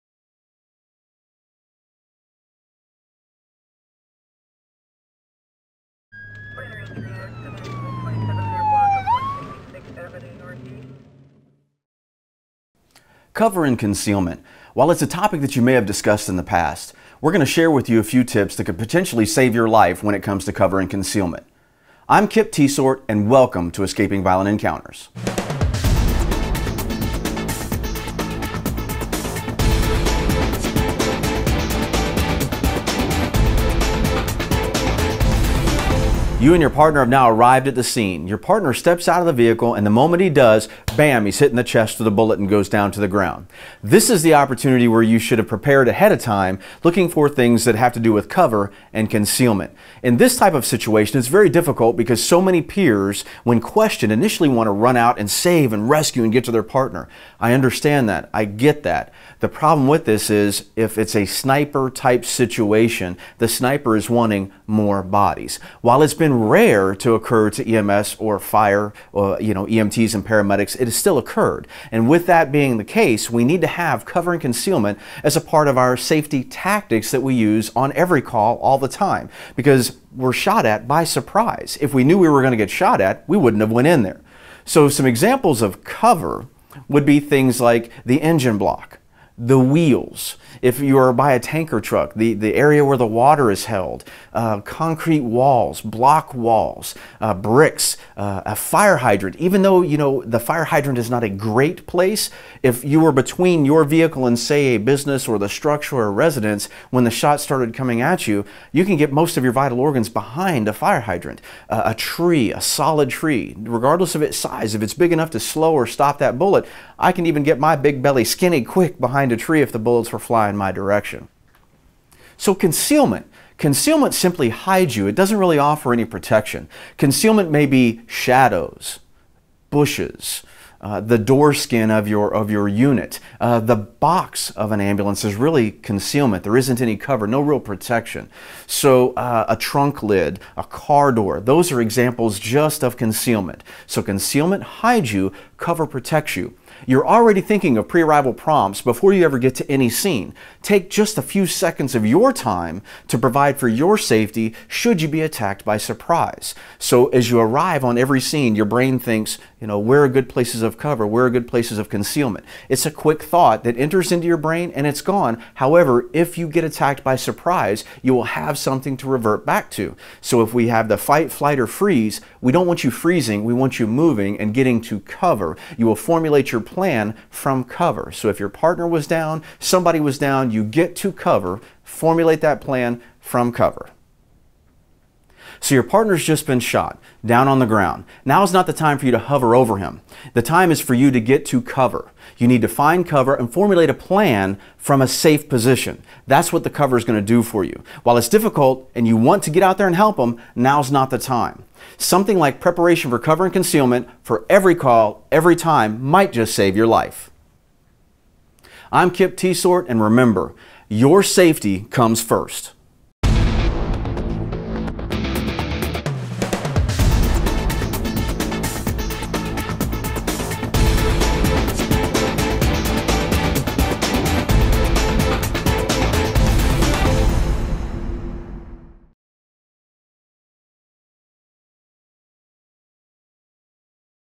Escaping Violent Encounters - EMS Defensive Tactics #7 - On Scene Cover and Concealment
Do you know the difference between cover and concealment?  What would you do if your partner was shot as you arrived on a scene?  Expert Kip Teitsort talks about the difference, and the importance of planning ahead for deadly encounters.  EMS scene safety starts with you.